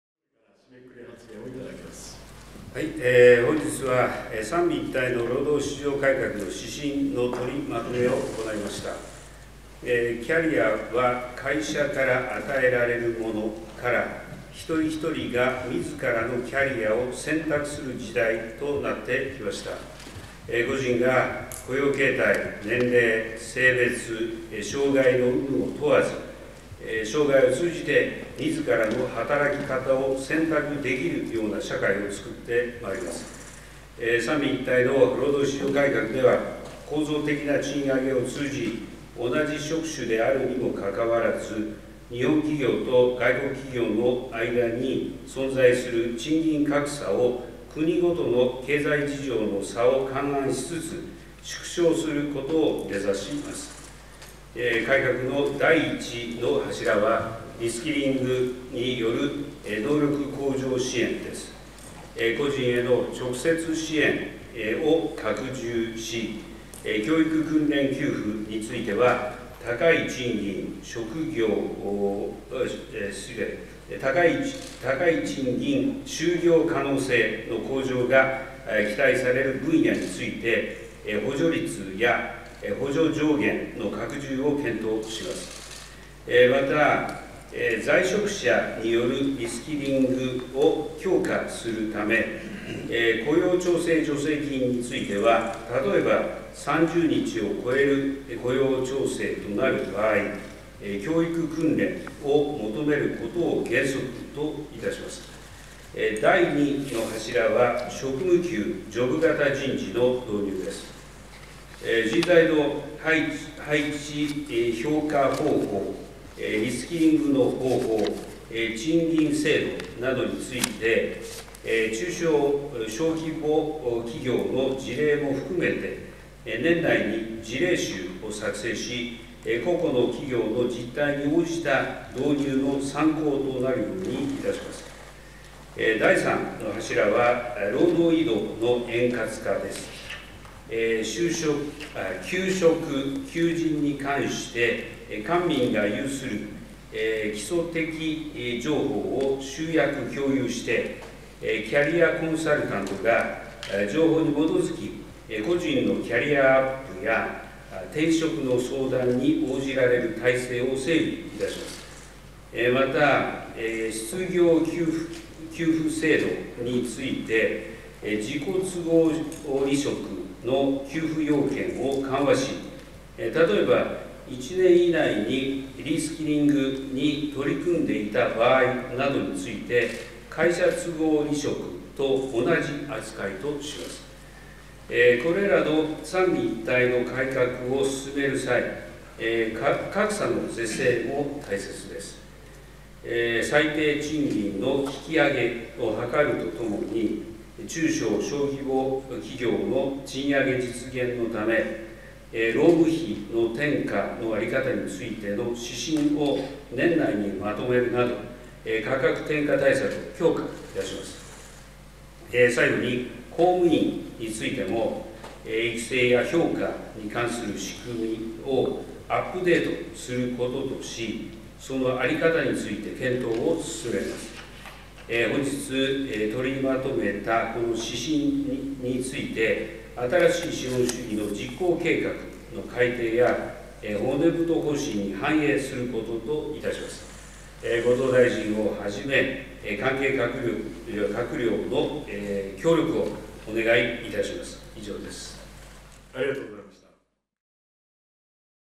新しい資本主義実現会議 ー令和5年5月16日
令和５年５月１６日、岸田総理は、総理大臣官邸で第１８回新しい資本主義実現会議を開催しました。

会議では、三位一体労働市場改革の指針（案）について、議論が行われました。

「本日は、三位一体の労働市場改革の指針の取りまとめを行いました。
　キャリアは会社から与えられるものから、一人ひとりが自らのキャリアを選択する時代となってきました。個人が、雇用形態、年齢、性別、障害の有無を問わず、生涯を通じて自らの働き方を選択できるような社会を作ってまいります。
　三位一体の労働市場改革では、構造的な賃上げを通じ、同じ職種であるにも関わらず、日本企業と外国企業の間に存在する賃金格差を、国ごとの経済事情の差を勘案しつつ、縮小することを目指します。
　改革の第１の柱は、リ・スキリングによる能力向上支援です。個人への直接支援を拡充し、教育訓練給付については、高い賃金・就業可能性の向上が期待される分野について、補助率や補助上限の拡充を検討します。また、在職者によるリ・スキリングを強化するため、雇用調整助成金については、例えば３０日を超える雇用調整となる場合、教育訓練を求めることを原則といたします。
　第２の柱は、職務給、ジョブ型人事の導入です。人材の配置・評価方法、リ・スキリングの方法、賃金制度などについて、中小・小規模企業の事例も含めて、年内に事例集を作成し、個々の企業の実態に応じた導入の参考となるようにいたします。
　第３の柱は、労働移動の円滑化です。求職・求人に関して官民が有する基礎的情報を集約、共有して、キャリアコンサルタントが、情報に基づき、個人のキャリアアップや転職の相談に応じられる体制を、整備いたします。また、失業給付制度について、自己都合離職の給付要件を緩和し例えば１年以内にリ・スキリングに取り組んでいた場合などについて、　会社都合離職と同じ扱いとします。
　これらの三位一体の改革を進める際、格差の是正も大切です。最低賃金の引上げを図るとともに、中小・小規模企業の賃上げ実現のため、労務費の転嫁の在り方についての指針を年内にまとめるなど、価格転嫁対策を強化いたします。
　最後に、公務員についても、育成や評価に関する仕組みをアップデートすることとし、その在り方について、検討を進めます。
　本日取りまとめたこの指針について、新しい資本主義の実行計画の改訂や骨太方針に反映することといたします。後藤大臣をはじめ、関係閣僚の協力をお願いいたします。」